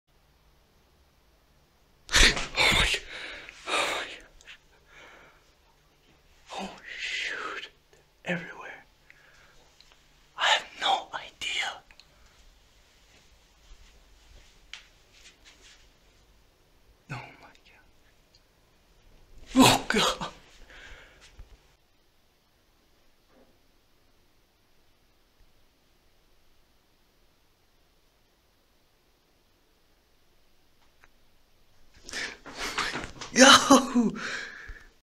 Bats in the house!
At 3am I woke up to around 50 bats flying above our bed! Our apartment is in the Czech Republic. We had to call the fire department.